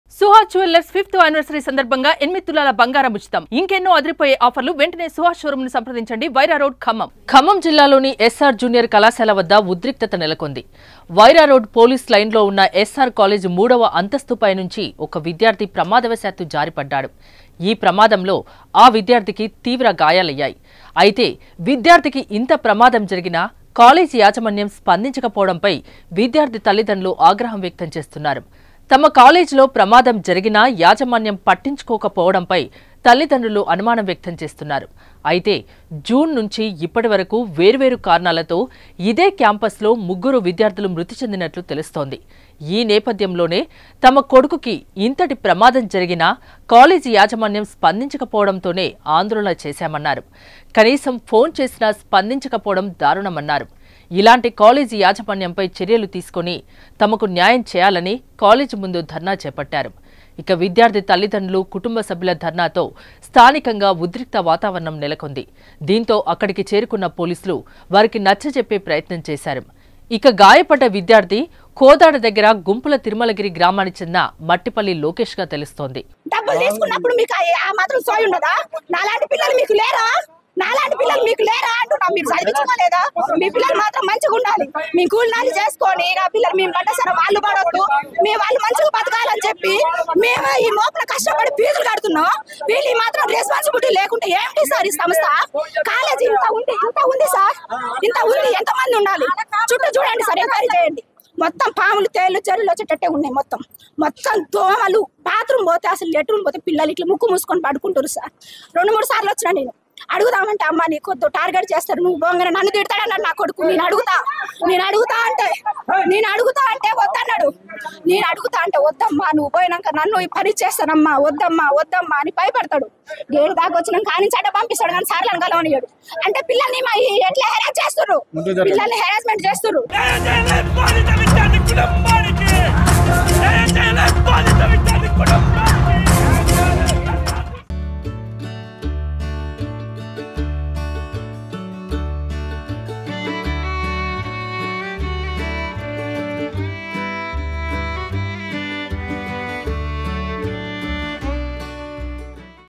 మూడవ అంతస్తు పై కింద పడ్డ ఇంటర్ విద్యార్థి..| Khammam SR Junior College Latest
Watch Here